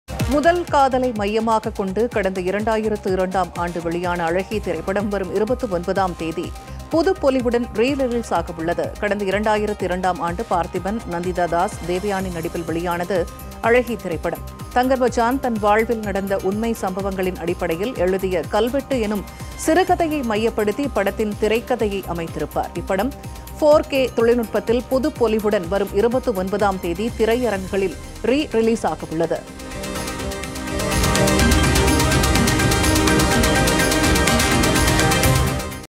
மார்ச் 29-ல் ரீரிலீஸ் ஆகும் ’அழகி 4K தொழில்நுட்பத்தில் வெளியாகிறது! | Re release | Jaya plus
மார்ச் 29-ல் ரீரிலீஸ் ஆகும் ’அழகி 4K தொழில்நுட்பத்தில் வெளியாகிறது! | Parthiban | Re release| Jaya plus